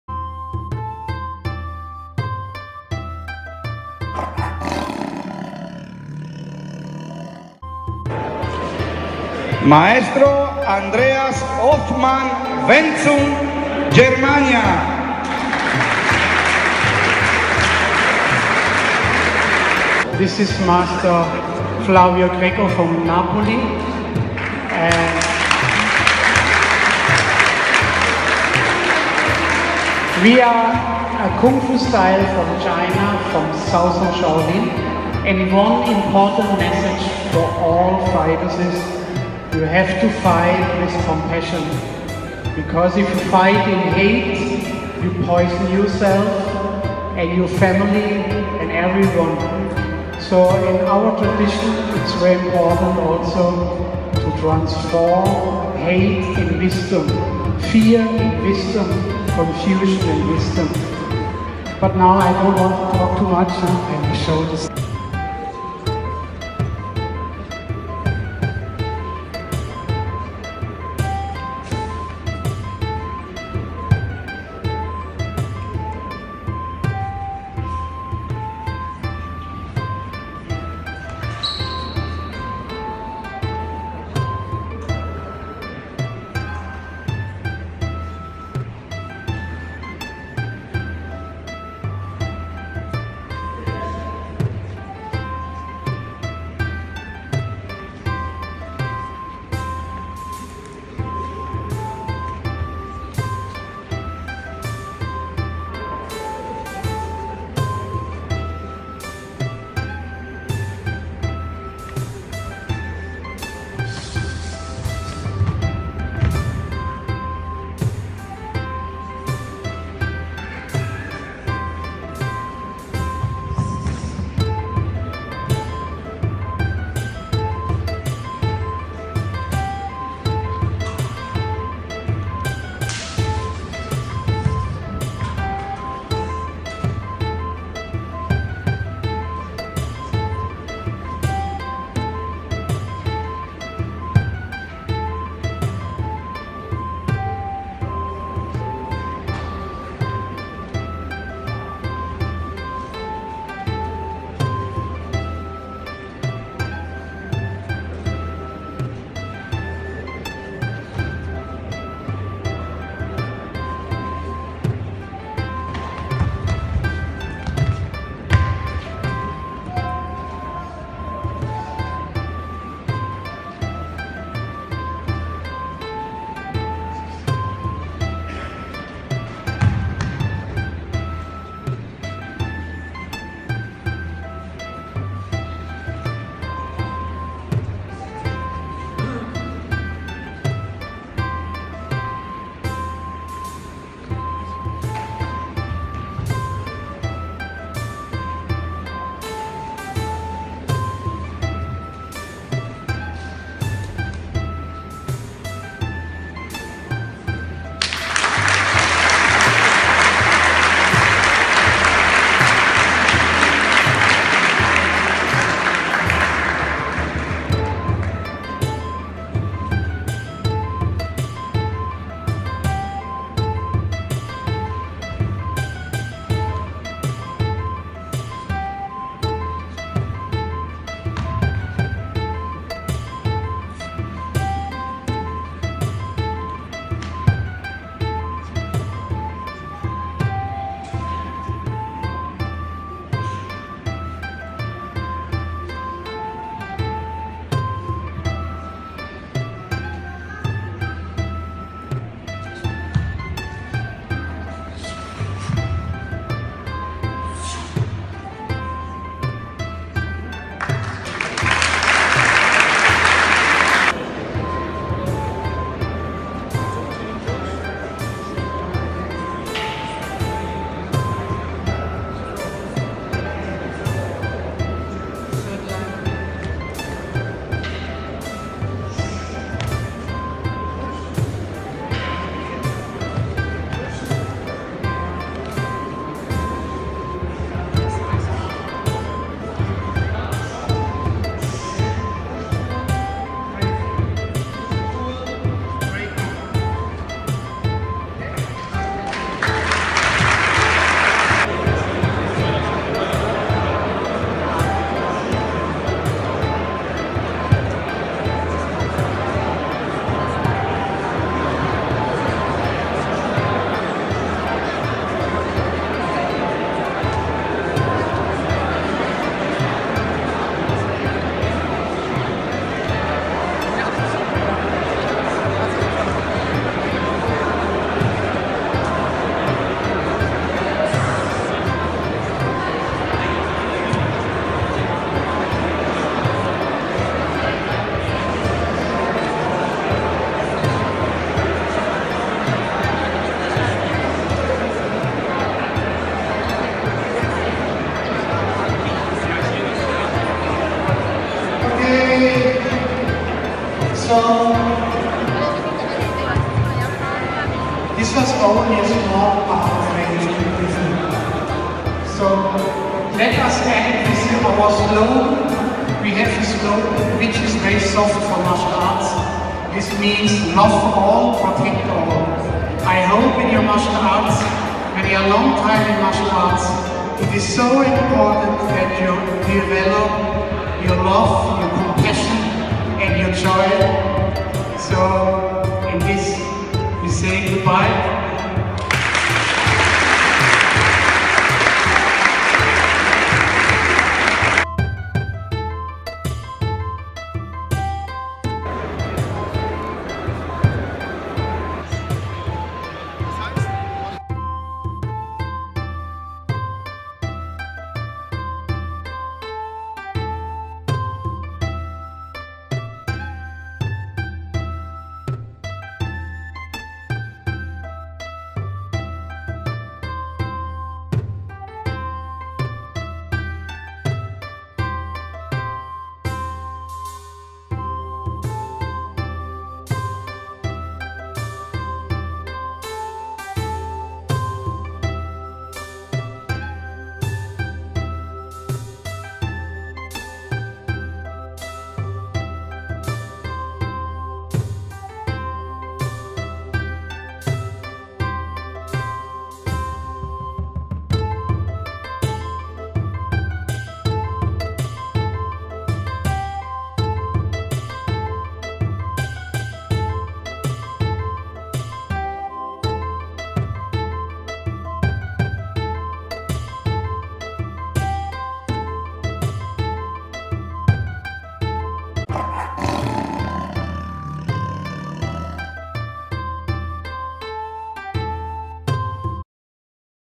Budo Masters 2016 Weng Chun Kung Fu demonstration
First World Meeting of Budo Masters 2016 in the eternal city and eternal spring Weng Chun family under the leadership of Grandmaster Andreas Hoffmann and Master Flavio Greco was invited to teach Shaolin Chi Sim Weng Chun Kung Fu there. In the video you can see the demonstration and weng chun meeting with other masters from wing chun,meet kune do, eskrima, Krav Maga, shaolin, karate, sami, systema, knife fighting etc.
erstes Treffen der Weltweiten Budo Meister in der ewigen Stadt Rom. Großmeister Andreas Hoffmann und Meister Flavio Greco wurden eingeladen Weng Chun Kung Fu zu unterrichten. Im Video wird die Demonstration gezeigt und das Treffen mit Meistern aus dem wing chun,meet kune do, eskrima, Krav Maga, shaolin, karate, sami, systema, Messerkampf etc.